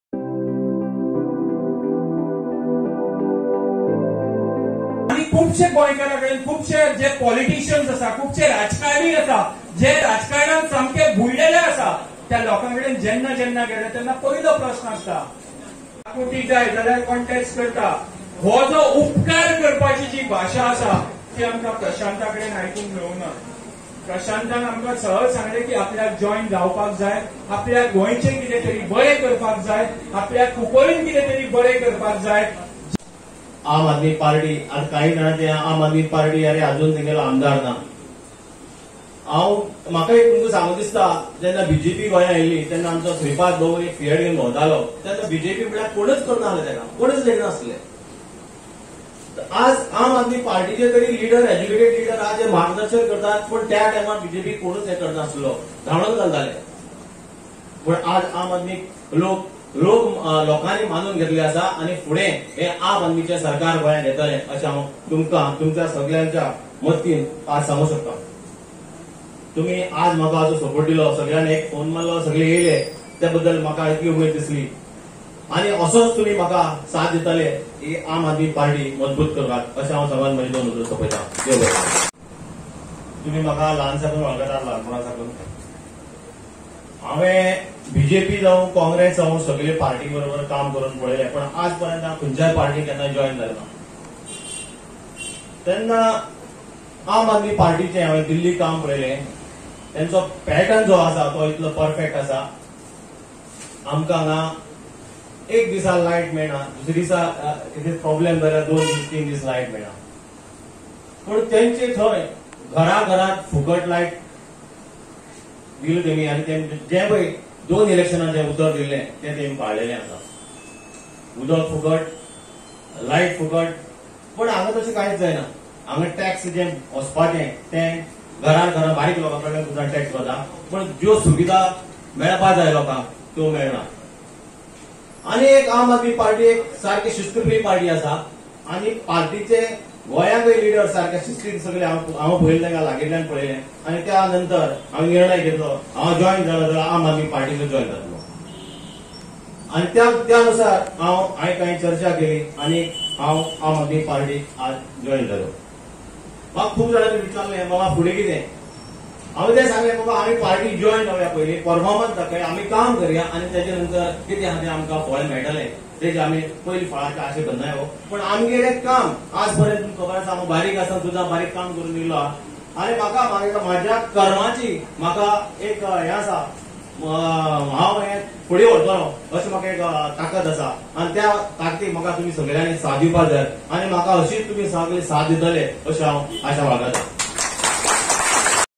AAP tries to fill Elvis Gomes huge hole by roping in Prashant Naik for Cuncolim constituency
Prashant Naik Balli managed to put up a good show, displaying show of strength during his induction in the party, indicates that he will be the face of the party in Cuncolim constituency, if not the party candidate in 2022.

Party national leader Durgesh Pathak, who had inducted Prashant in the party in the presence of other party leaders, said the party will decide on the ticket and the candidate before the election. “Many people are joining the party to lend their hand to build and strengthen the party. Party tickets will be decided at a later stage”, he added.

When a scribe made a mention of ex-AAP convenor Elvis Gomes, Pathak said the party had no issues with the outgoing party chief, who had quit the party in December last. He said the doors of the party are open for anyone, including those who had left the party and those intending to join the organization.